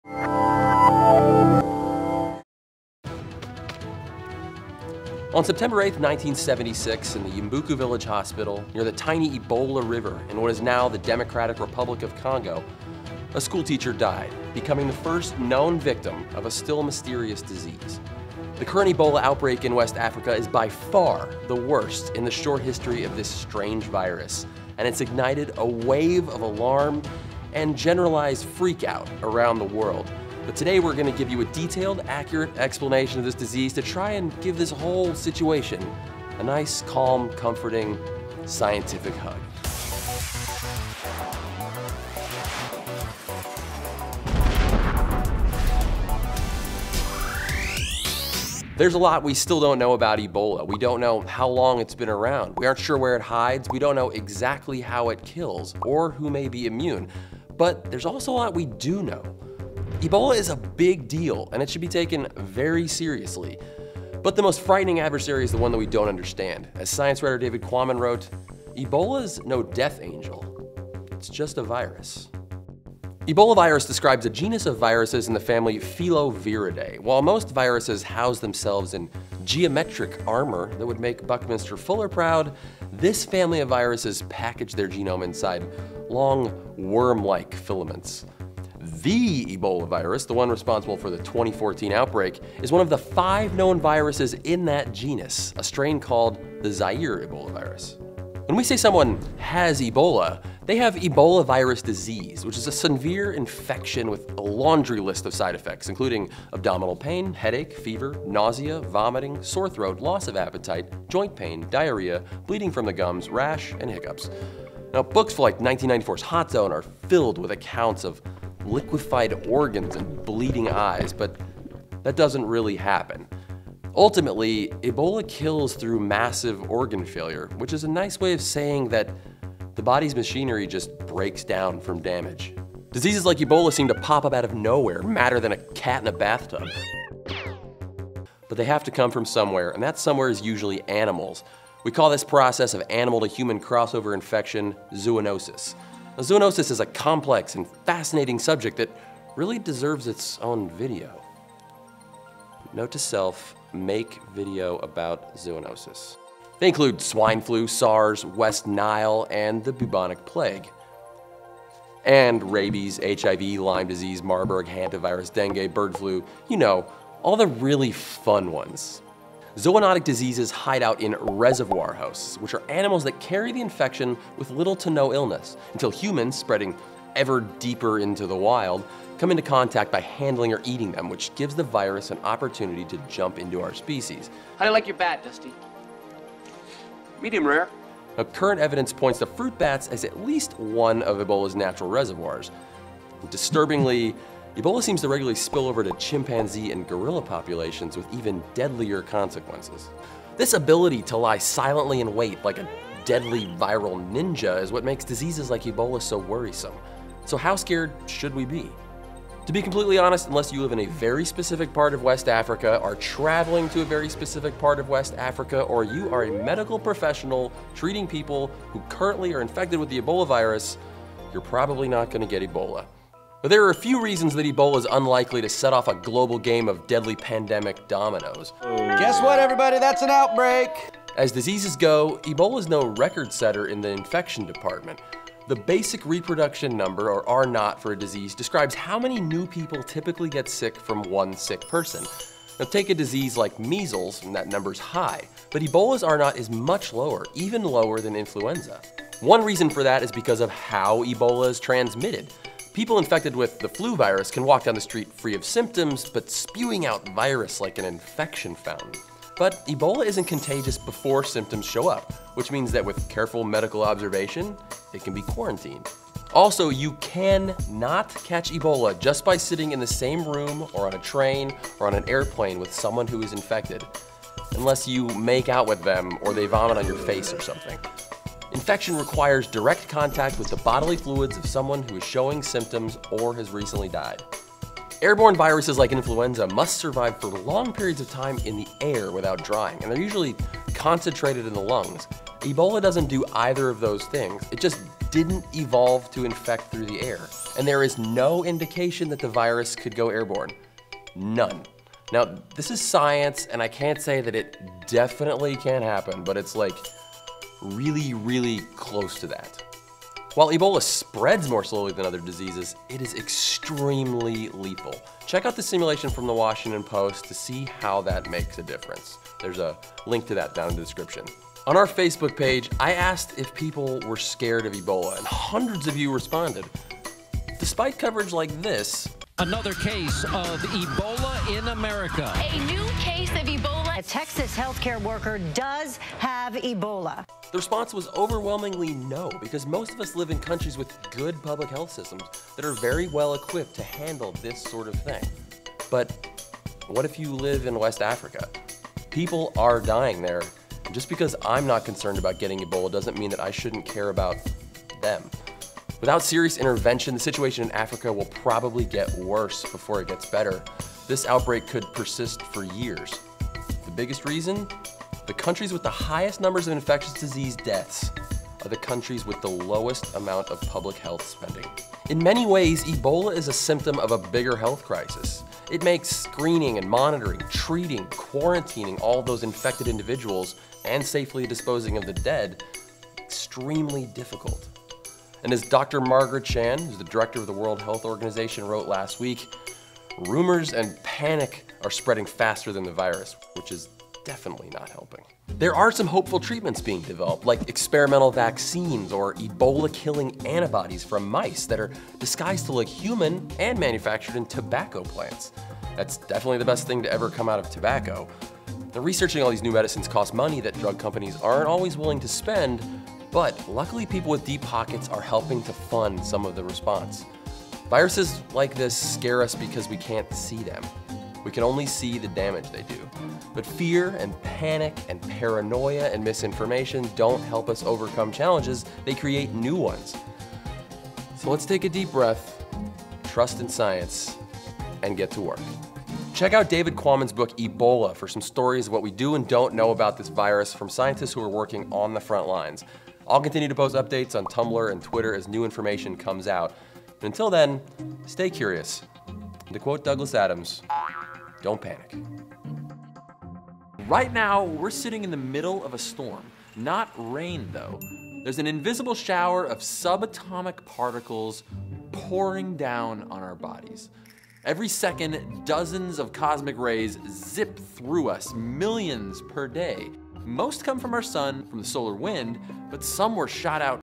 Ebola Explained
What you need to know about one of nature's scariest viruses.

Read David Quammen's "Ebola"

Read the original 1976 case report describing the first Ebola patient

Can Ebola go airborne? No.

Joe Hanson - Host and writer
Joe Nicolosi - Director
Amanda Fox - Producer, Spotzen IncKate Eads - Associate Producer
Katie Graham - Director of Photography
Graham Hutchins - Editing/Motion Graphics

Stock images via Shutterstock